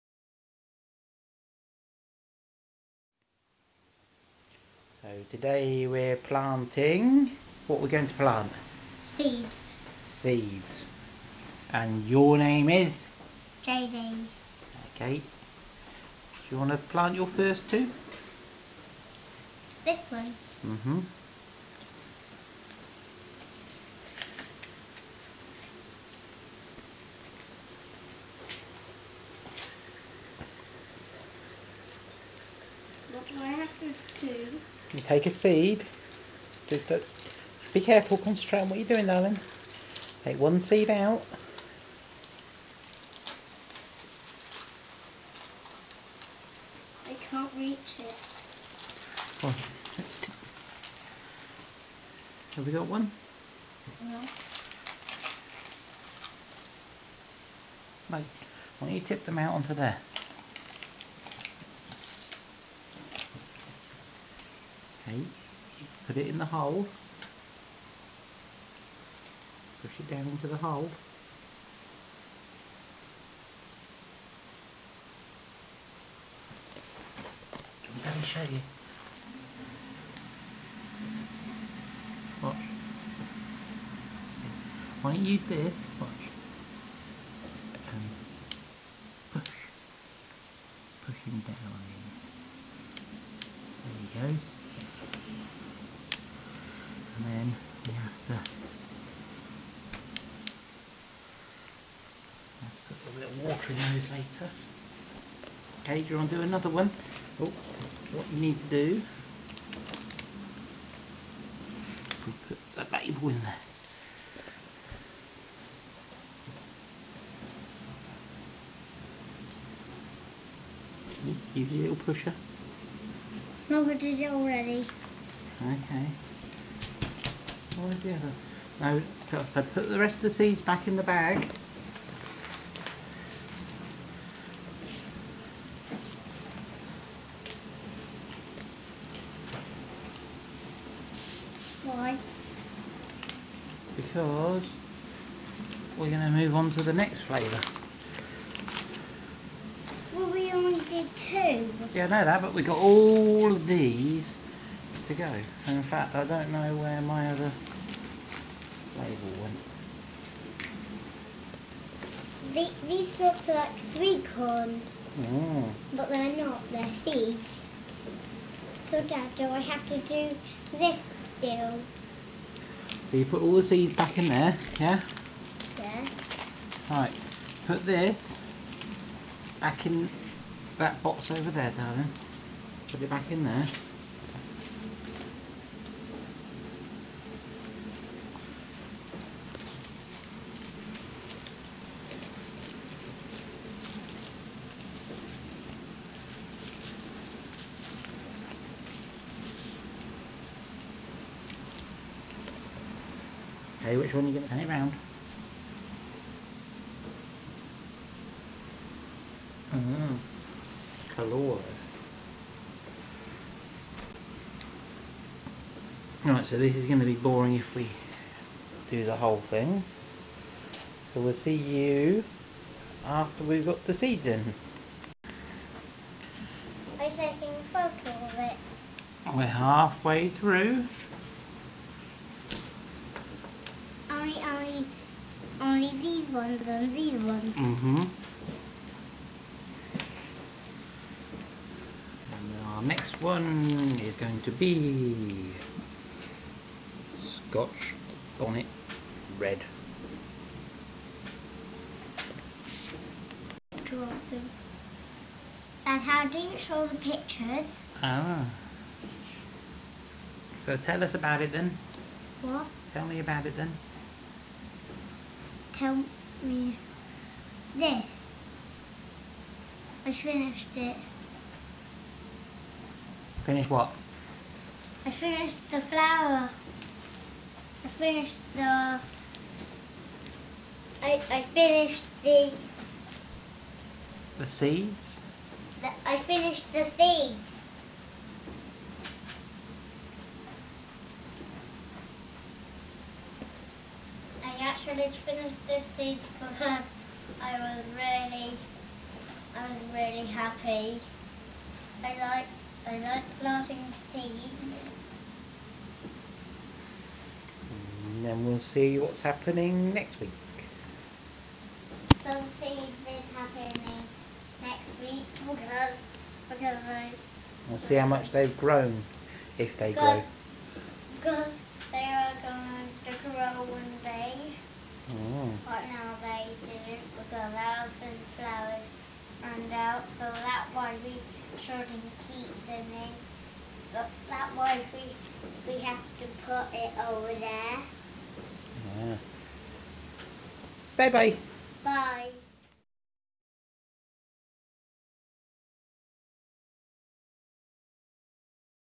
Chilli Growing - Week 1 - The Beginning
With the help of JD we planted our first batch of chilli seeds.  I know it's a bit late to be planting but it's fun for the kids.  You never know, they might grow.

Seeds planted:  Navaho, Numex Bailey, Serrano, Jalapeno, Purple Tiger, Scotch Bonnet Red, Scotch Bonnet Orange, Hungarian Hot Wax, Ring O Fire, Habanero Red, Caloro, Dorset Naga.

Only 2 seeds of each variety planted.  Lots of seeds left and a few varieties left to sow.